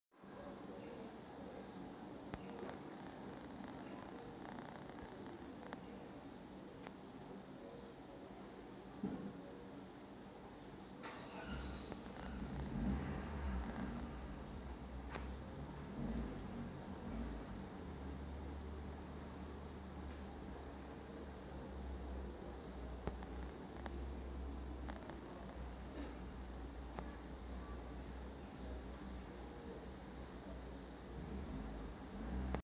Macro lens test on forficula auricularia with Nexus 6P on 720p
Macro lens testing on Nexus 6P